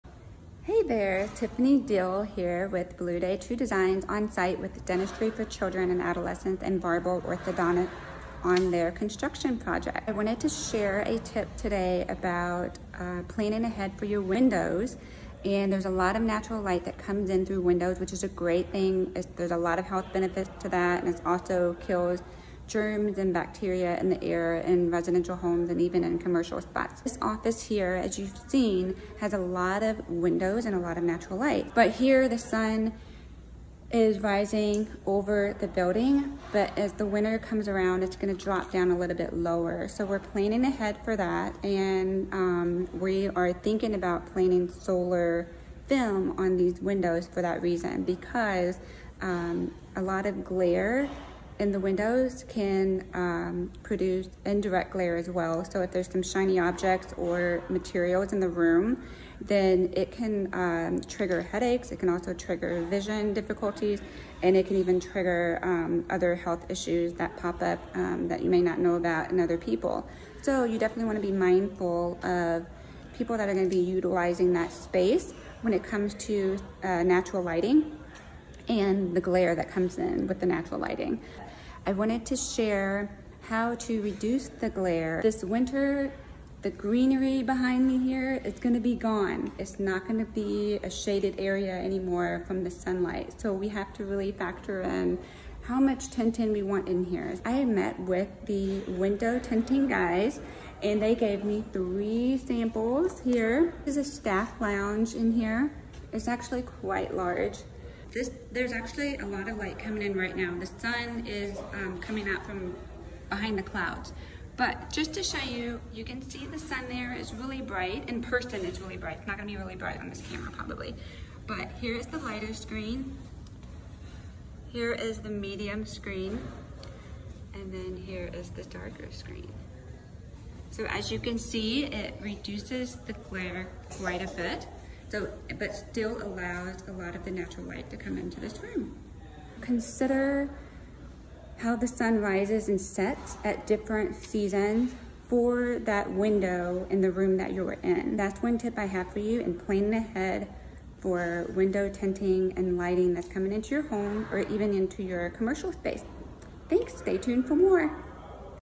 Tuesday Tip: Window Treatment
Things to consider when planning ahead for window treatment and tinting.